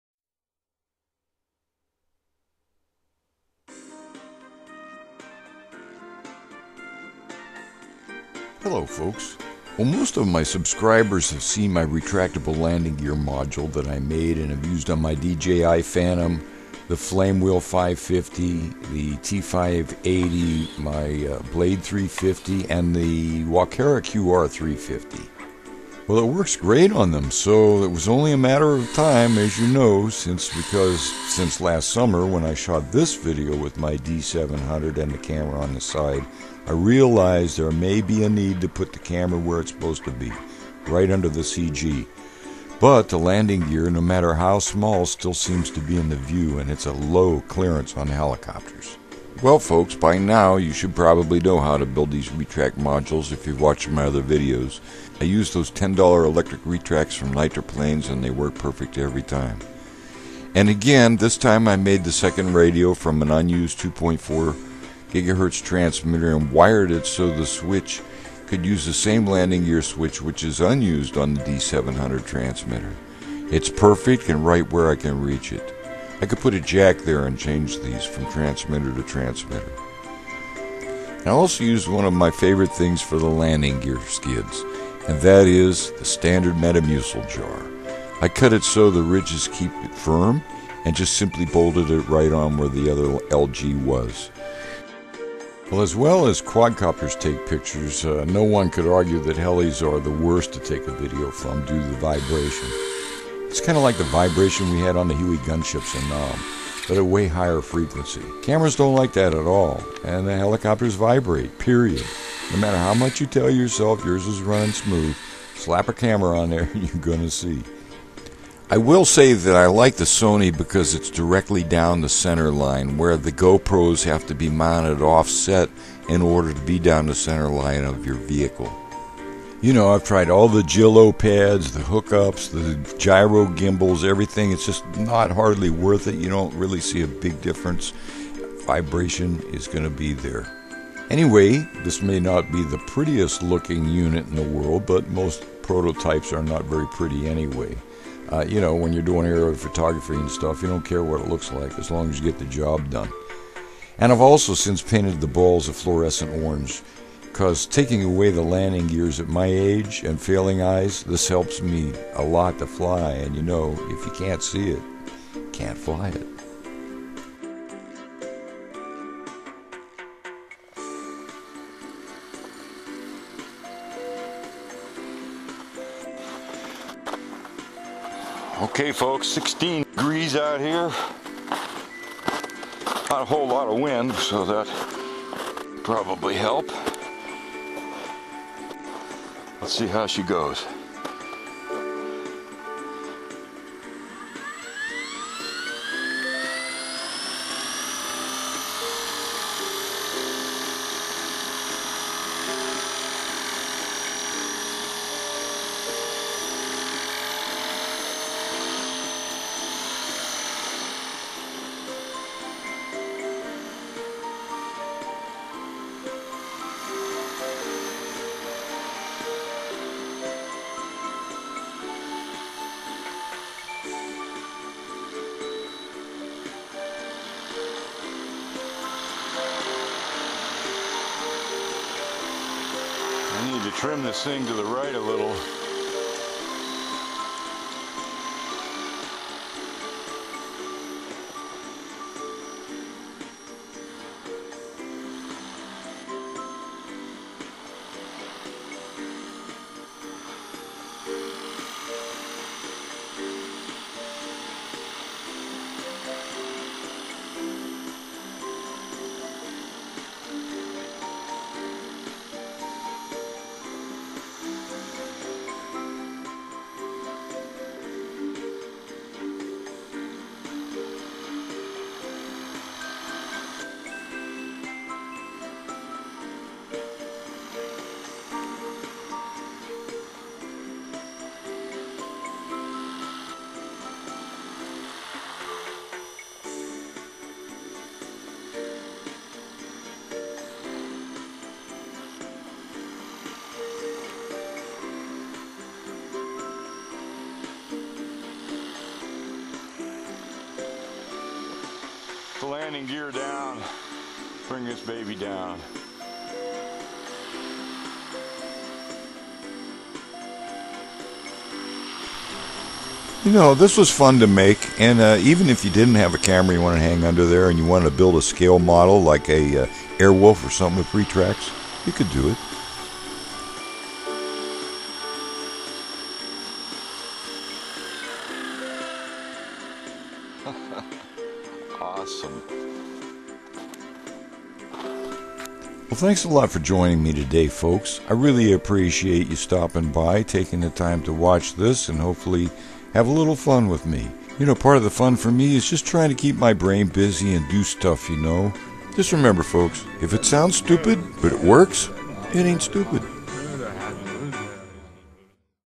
Esky D-700 Helicopter with Retractable Landing Gear-Camera Module. by NightFlyyer.
Taking the experience I learned  from installing my retract module on my Quads and Hexrotored machines, I decided to make one for my D-700 3D helicopter this time.  Although helis inherently vibrate more than quads, they also have a very low ground clearance. For that reason not many folks use stock helis for camera platforms anymore since the advent of smooth multirotored machines.   Cameras hanging down from small helis, usually means low ground clearance with short landing gears.  Stock landing gears aren't too good either and need to  be higher or they are usually in the view of the wide angle cameras.  I made a simple retractable landing gear to eliminate that, plus it makes the unit look better and more aerodynamic.  Many different ways to do this can be done and with the advent of the inexpensive electric retracts from Nitroplanes, even a scale Airwolf can be had.  I am hoping this inspires you to design your own.

Dave Herbert
Academy of Model Aeronautics
AMA L-8221 Life Member
Contest Director/Scientific